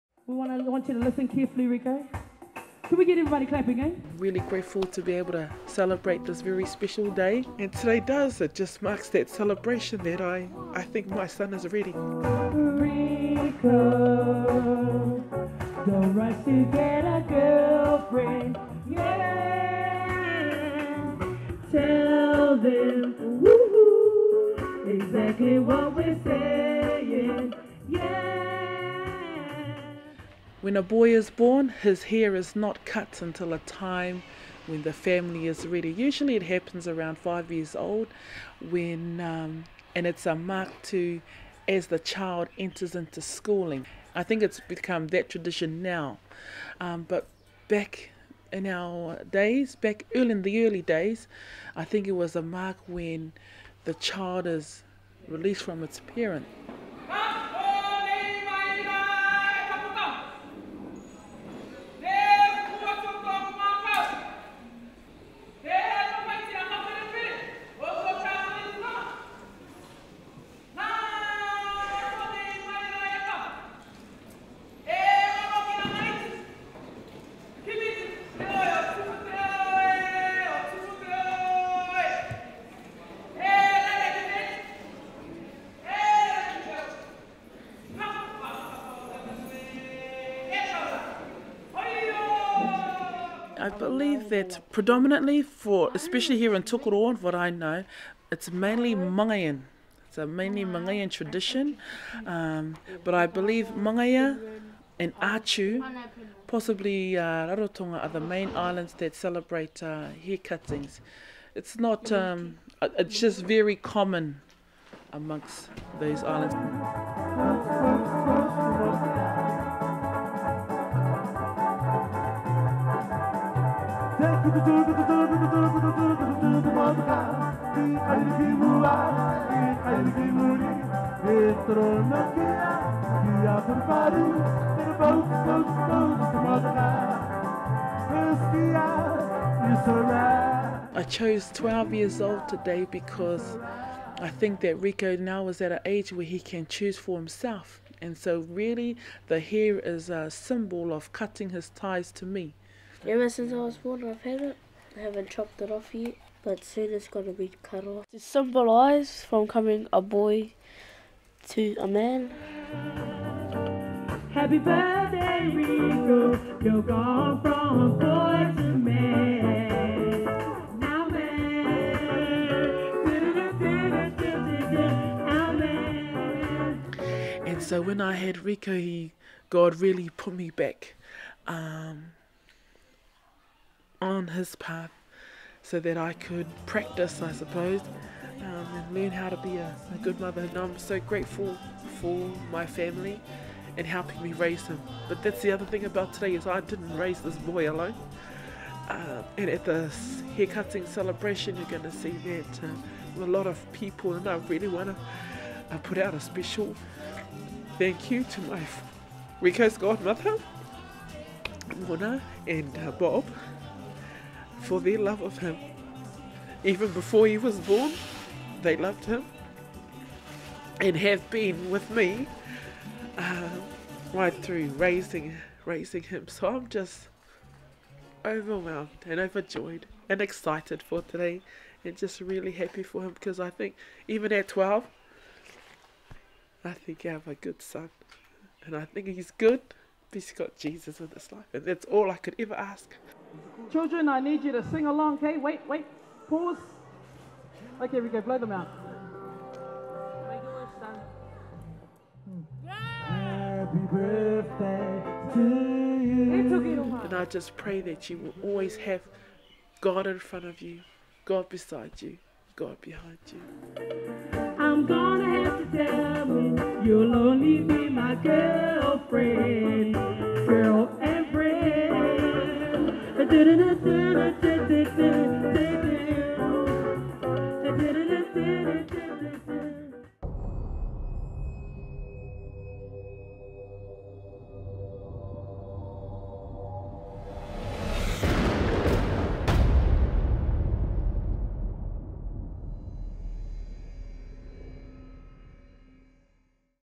Rico's Haircutting & 12th Birthday Celebration
Short movie about Cook Island Haircutting in Tokoroa